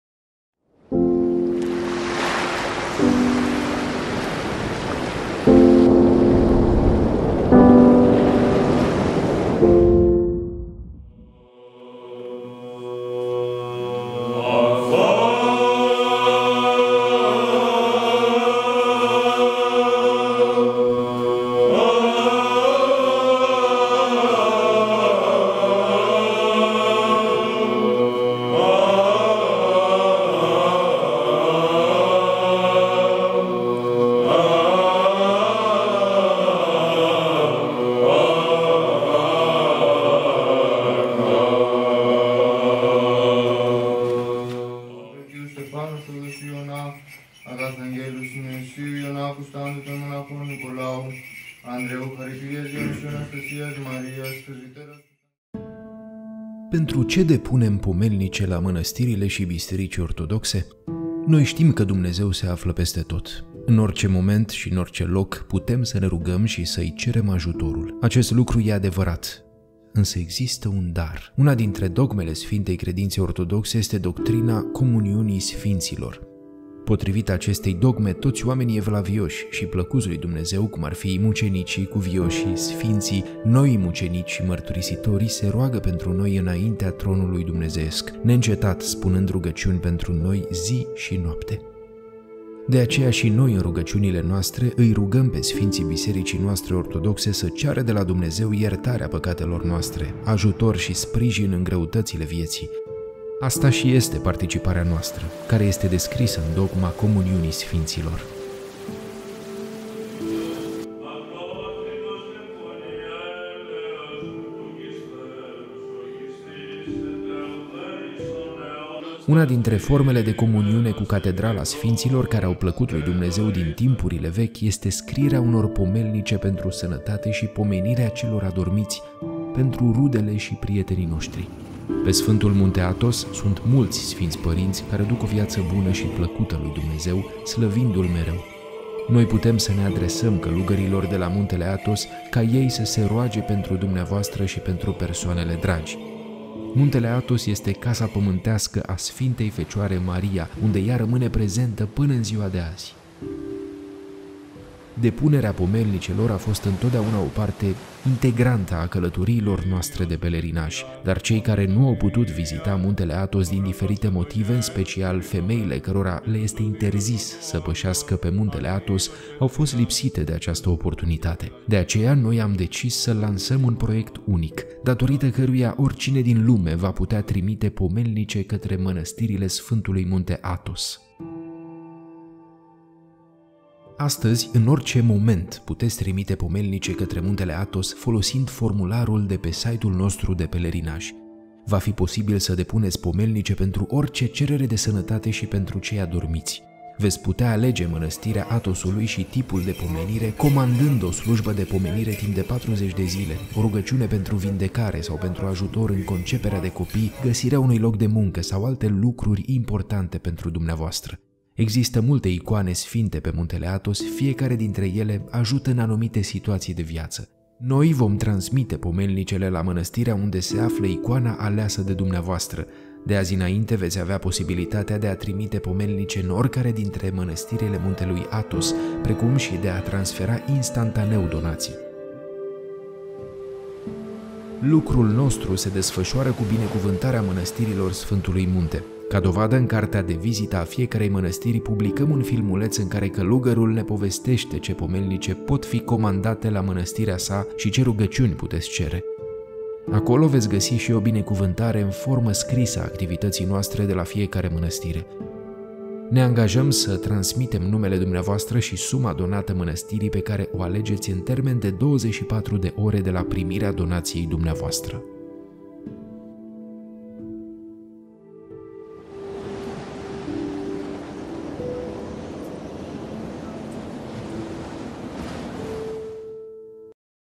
Depune pomelnice pentru sănătate și răposați la mănăstirile de pe Sfîntul Munte Athos
Pentru ce depunem pomelnice la mănăstirile și bisericile Ortodoxe?
Una dintre dogmele sfintei credințe ortodoxe este doctrina "Comuniunii Sfinților". Potrivit acestei dogme, toți oamenii evlavioși și plăcuți lui Dumnezeu, cum ar fi mucenicii, cuvioșii, sfinții, noii mucenici și mărturisitorii, se roagă pentru noi înaintea Tronului Dumnezeiesc, neîncetat spunând rugăciuni pentru noi, zi și noapte. De aceea și noi în rugăciunile noastre, îi rugăm pe sfinții Bisericii noastre ortodoxe, să ceară de la Dumnezeu iertarea păcatelor noastre, ajutor și sprijin în greutățile vieții.
Pe Sfântul Munte Athos sunt mulți sfinți părinți care duc o viață bună și plăcută lui Dumnezeu, slăvindu-L mereu. Noi putem să ne adresăm călugărilor de la Muntele Athos ca ei să se roage pentru dumneavoastră și pentru persoanele dragi.
Astăzi, în orice moment puteți trimite pomelnice către Muntele Athos, folosind formularul de pe site-ul nostru de pelerinaj. Va fi posibil să depuneți pomelnice pentru orice cerere de sănătate și pentru cei adormiți. Veți putea alege mănăstirea Athosului și tipul de pomenire, comandând o slujbă de pomenire timp de 40 zile, o rugăciune pentru vindecare sau pentru ajutor în conceperea de copii, găsirea unui loc de muncă sau alte lucruri importante pentru dumneavoastră.
Noi am creat acest proiect pentru ca și dvs. să le puteți cere să se roage pentru dvs. și pentru cei dragi vouă la Muntele Athos, pământul sfânt al Maicii Domnului, unde Maica Domnului este prezentă până în zilele noastre.

Magazin online cu produse realizate de călugării Sfântului Munte Athos.